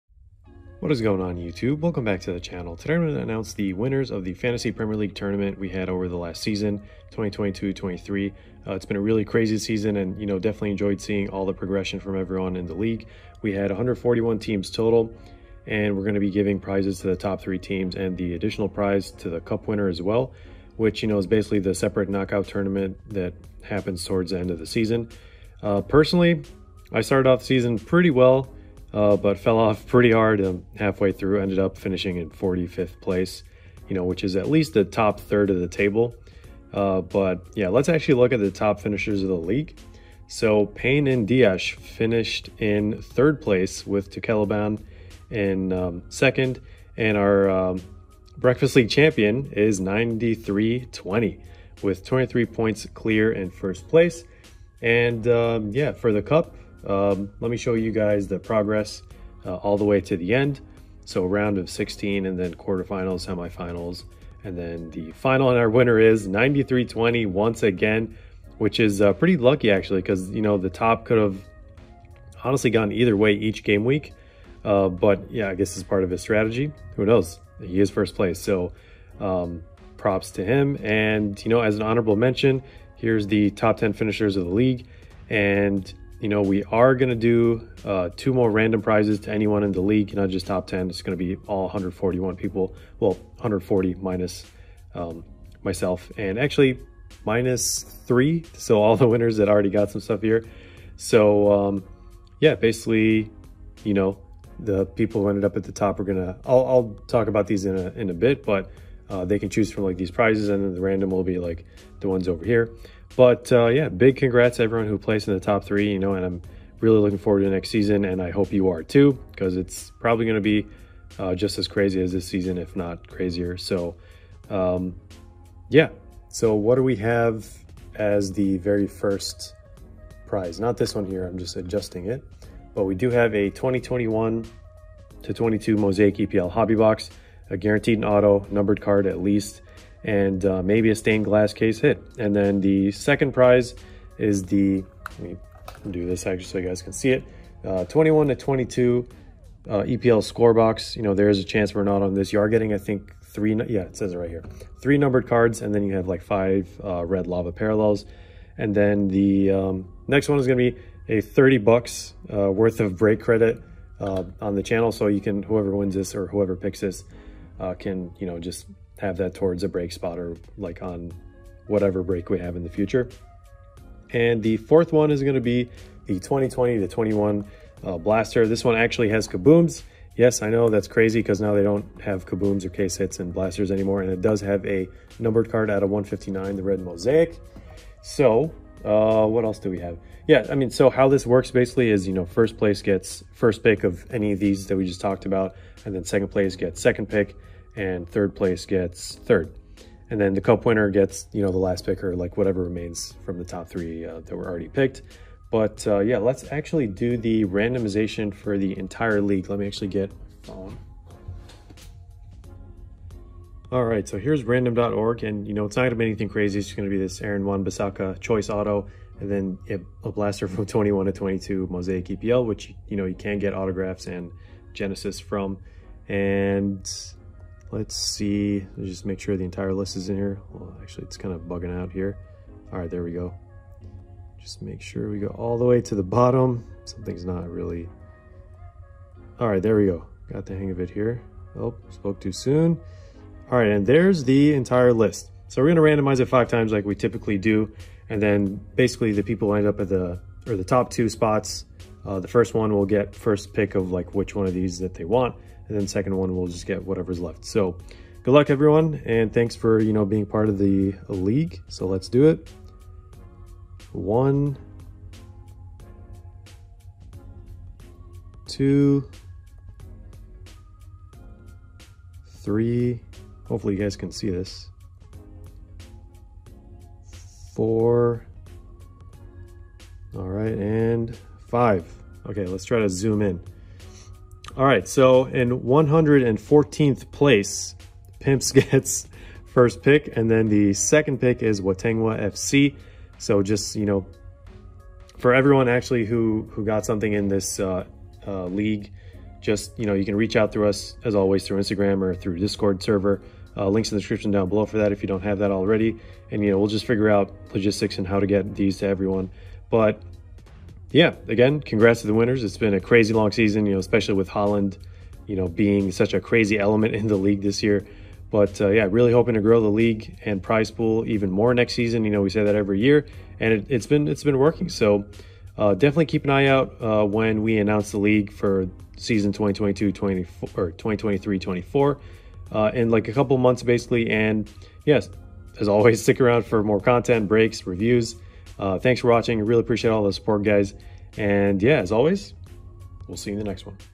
FPL Winners BREAKfast League 2022-23 Fantasy Premier League Soccer
In this video we announce the winners of the Fantasy Premier League BREAKfast League for the 2022-23 season. Top 3 winners and the Cup winner are eligible for prizes, with a randomized giveaway to everyone in the league.
Check it out and join us next season!

© Music copyright license agreement with Boss Soundstripe Productions.
Code: VVGHRSYKI6KLYP2J